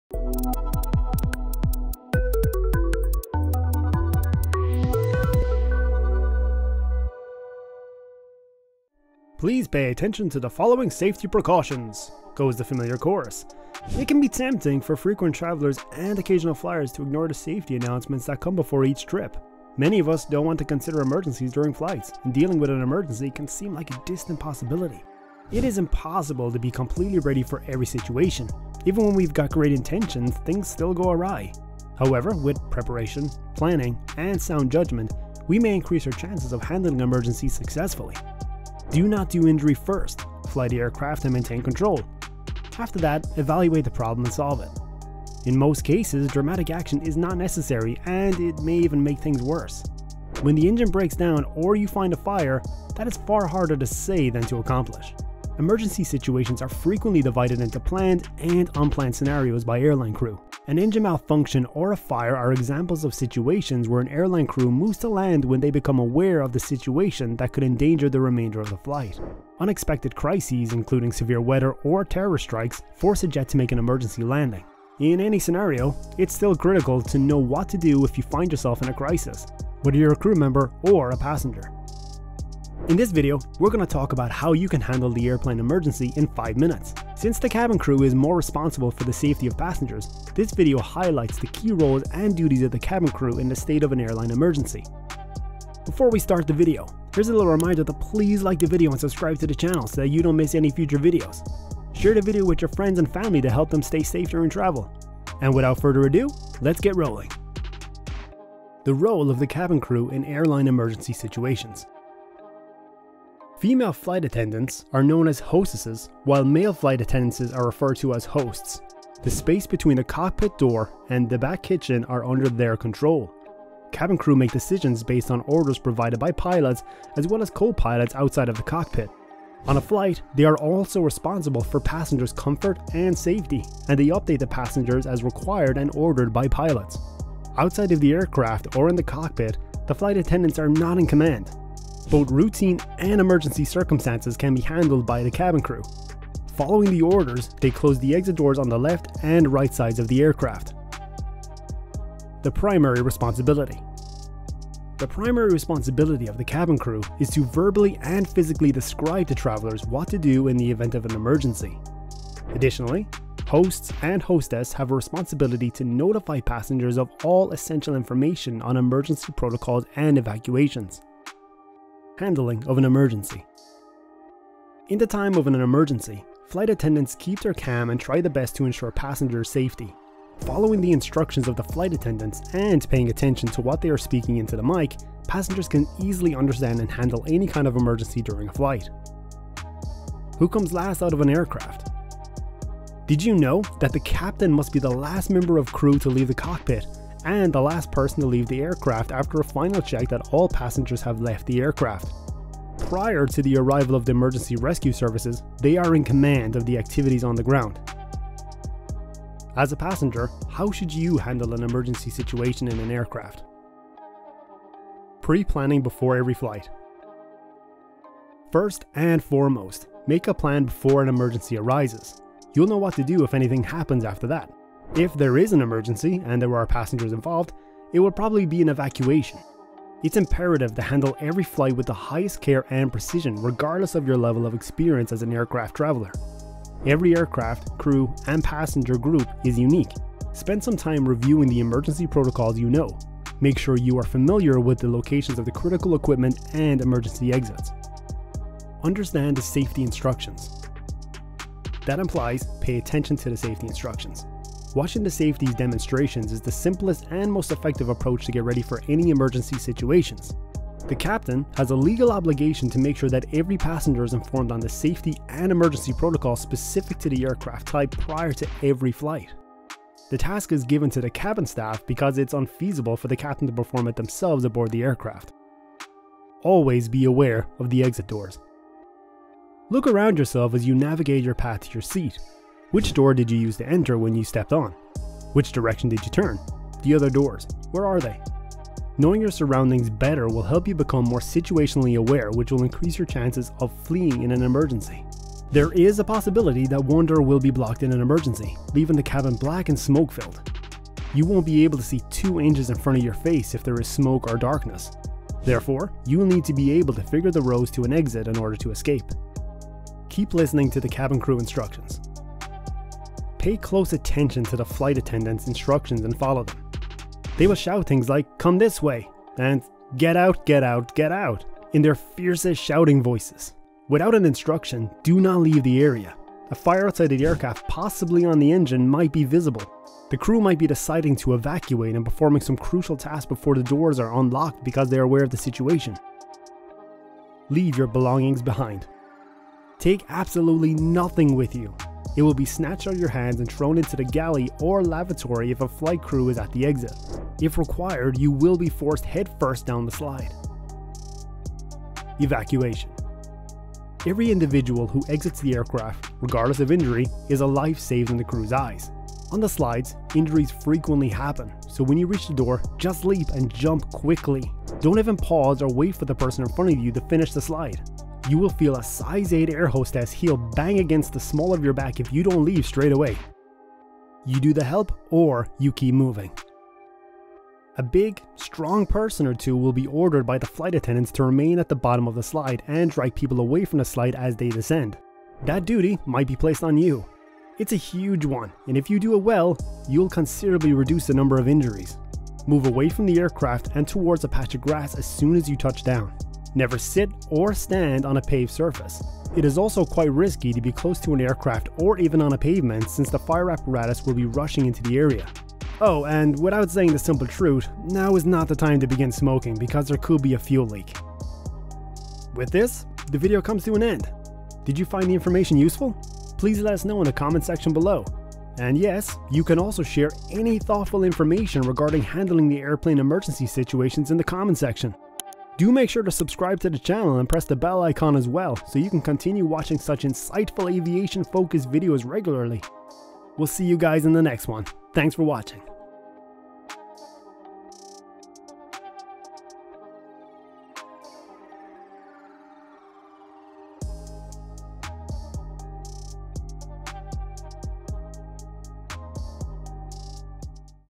Airplane EMERGENCY And What's Really Going On
Aviation - Cabin Crew Duties - And What Should You Do?
In this video we look at the duties are responsibilities after an Emergency occurs in an aircraft. How should you react and what should you do as a Passenger? More Often than not you will not need to worry about these situations, however, learning what really goes on in the cabin and cockpit is usefull for both you and the cabin crew. Do you know what to do in case of an evacuation or emergency landing?

How To Handle An Airplane Emergency & What Are The Duties of The Cabin Crew